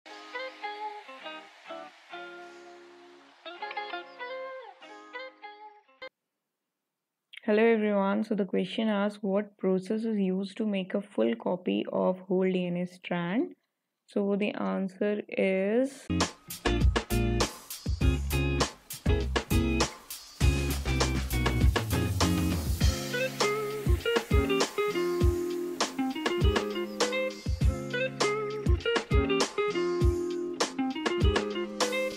1. What process is used to make a FULL COPY of the WHOLE DNA strand? 2. This is a DNA strand: TACGA…
1. What process is used to make a FULL COPY of the WHOLE DNA strand? 2. This is a DNA strand: TACGAACTG. What is the mRNA strand that would be made through transcription? 3. Which amino acids would be coded for by the mRNA strand in question 2?